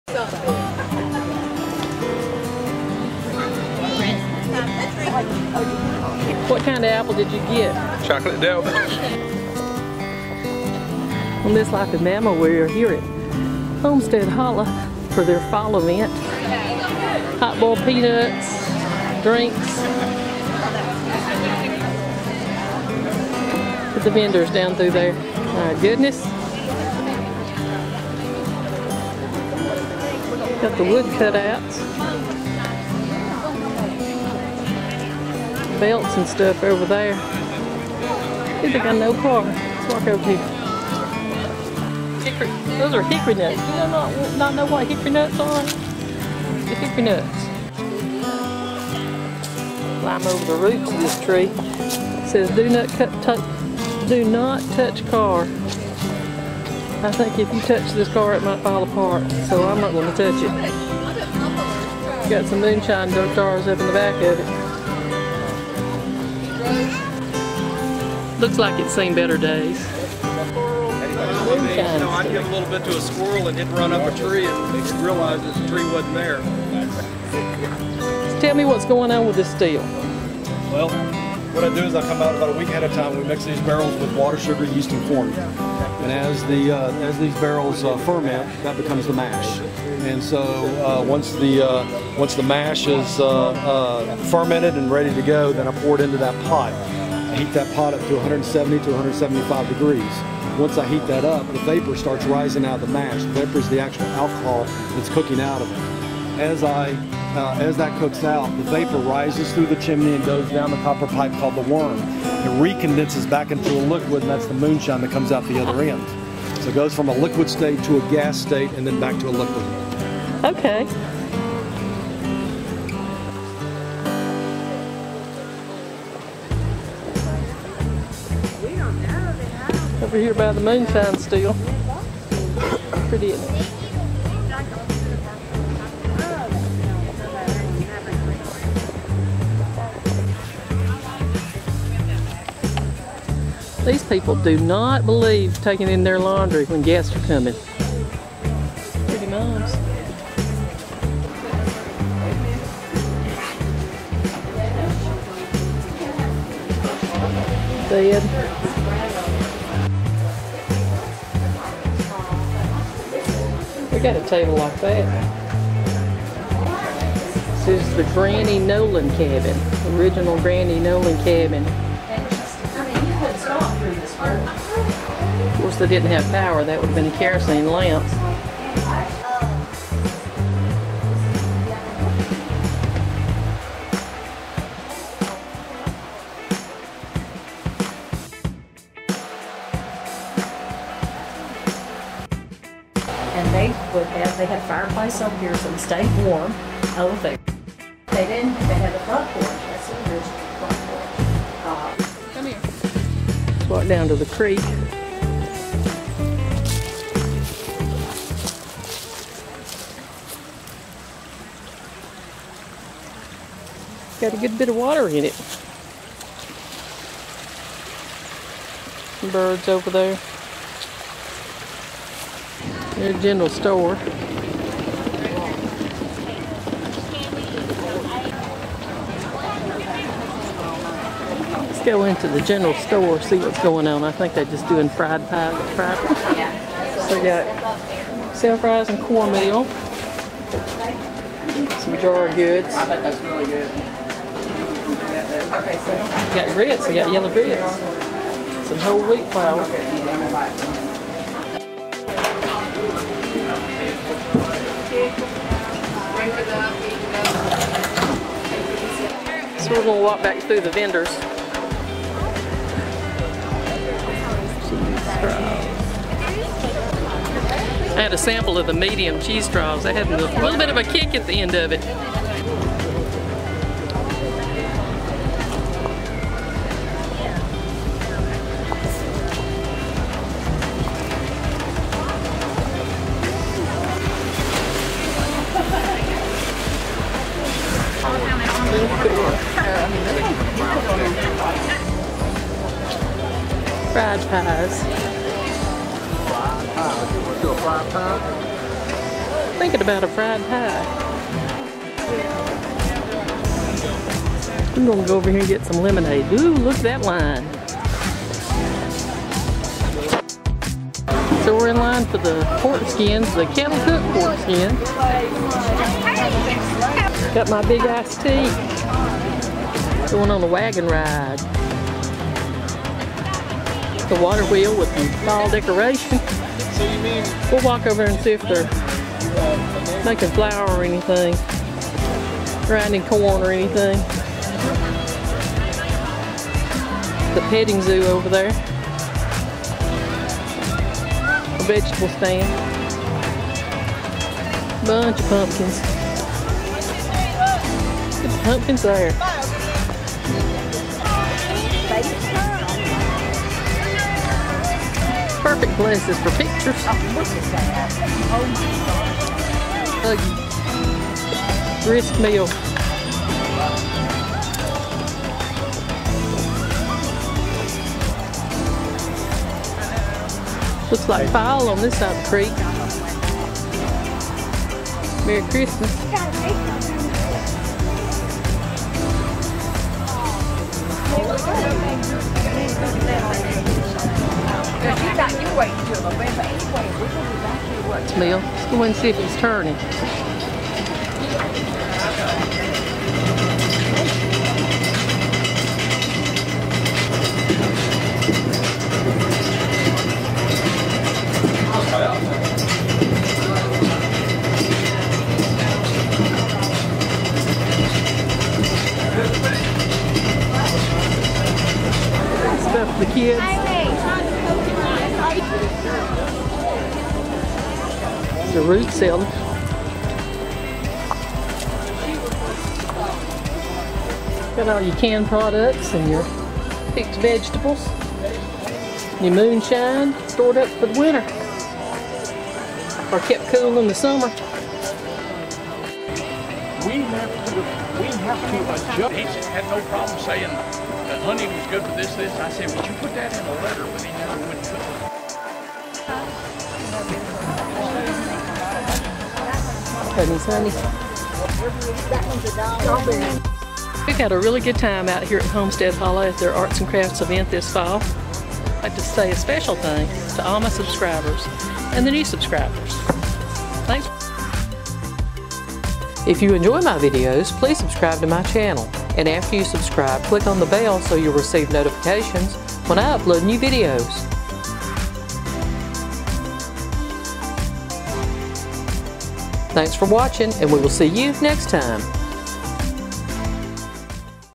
Homestead Hollow Fall Festival Part 2 with Life In Bama
On this Life In Bama we continue to enjoy our visit to the Homestead Hollow Fall Arts and Crafts Festival in Springville. It is a hot day but we are having a fun time looking in the old time cabins here at the hollow and browsing the booths. We even see a Moonshine Still!
Join us on our journey and this 2nd video from Homestead Hollow.